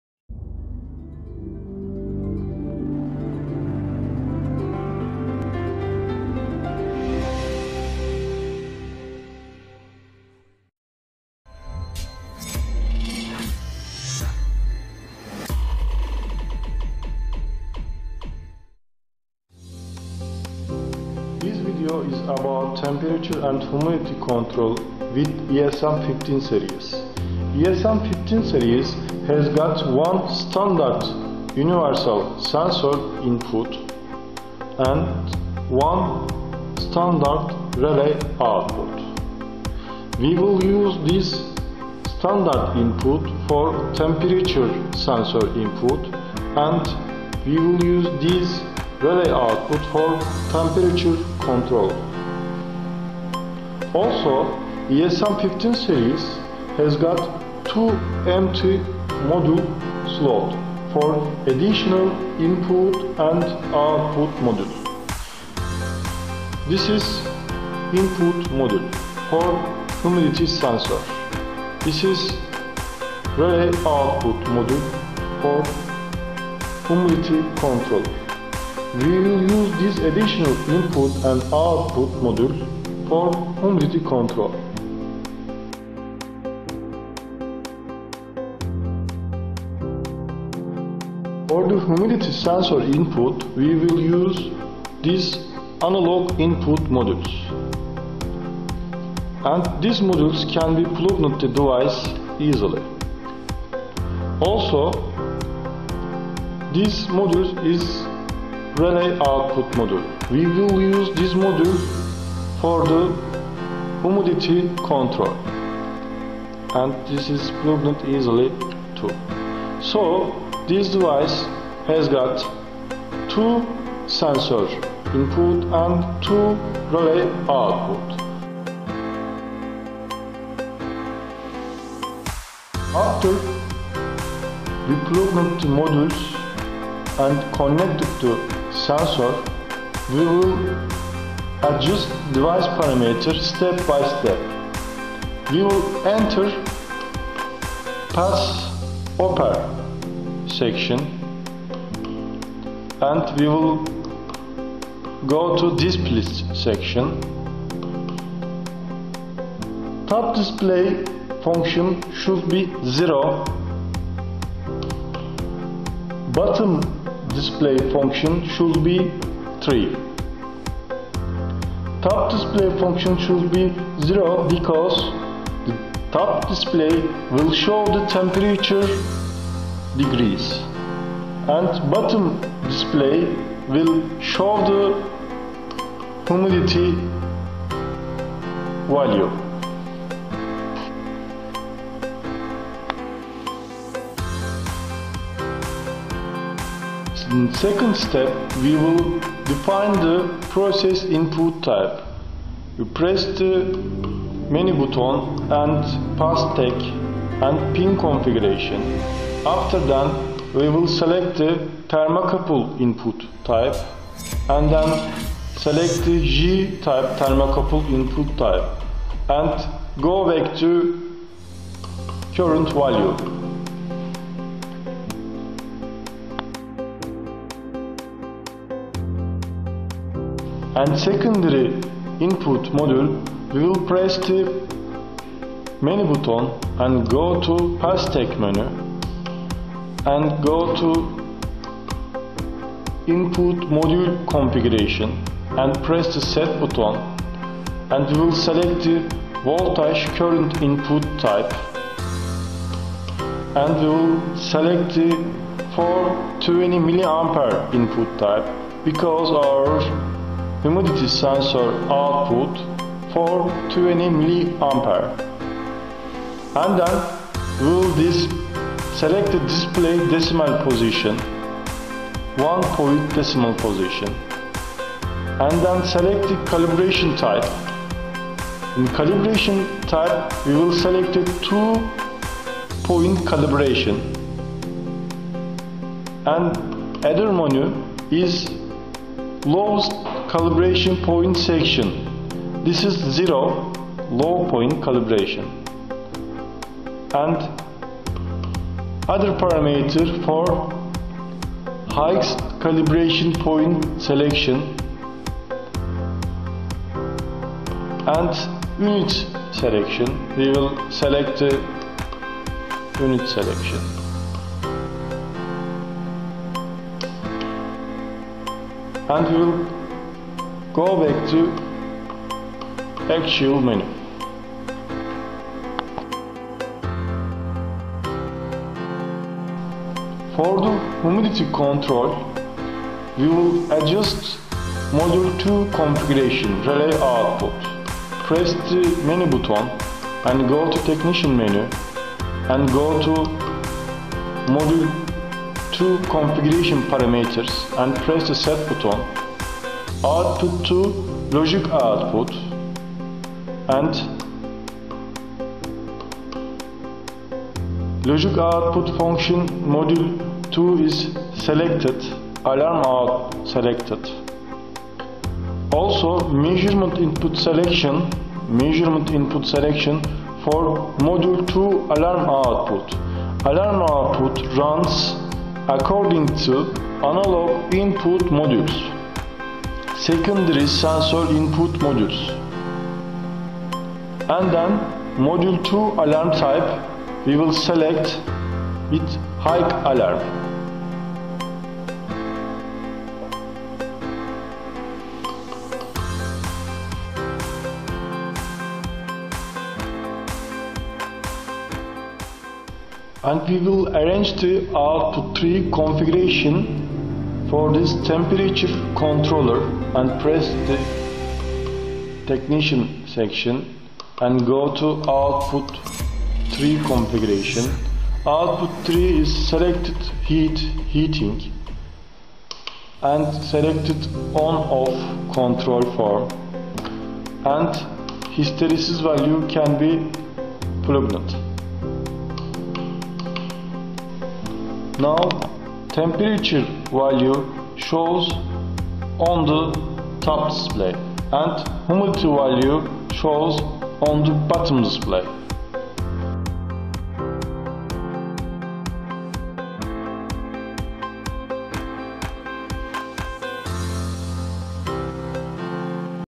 Emko Elektronik ESM-4450 Humidity and Temperature Control ESM-4950 , ESM-7750, ESM-9450, ESM-9950
ESM-XX50 'Smart IO Module' Process Controller
ESM-XX50 series process controllers can be used to process input value (temperature, humidity, weight, pressure etc.) and control desired outputs accordingly. The Input/Output modules, Universal Inputs, RS 232/ RS 485 serial communication modules boraden the application fields of ESM-XX50 series process controllers. Some of the applications can be listed as follows; PID Process Control, Motorized Valve Control, Profile Control,Heating Failure Detection.

For more detailed information about the product, please visit our or contact our company.

ESM-XX50 Üniversal Girişli PID Smart I/O Modül Sistemli Proses Kontrol Cihazı
ESM-XX50 serisi proses kontrol giriş değerlerini işlemek için kullanılır (sıcaklık, nem, ağırlık, basınç vb) ve kontrole uygun istenen çıkışlar kullanılabilir.Giriş / Çıkış modülleri, Üniversal Girişler, RS 232 / RS 485 seri haberleşme modülleri ESM-XX50 serisi proses kontrol cihazında uygulanabilir. Uygulamaların bazıları şöyle sıralanabilir; PID Proses Kontrol, Motorlu Vana Kontrolü, Profil Kontrol, Isıtma Arıza Tespiti v.b.
Ürün hakkında daha detaylı bilgi edinmek için web sitemizi ziyaret edebilir veya firmamız ile iletişime geçebilirsiniz.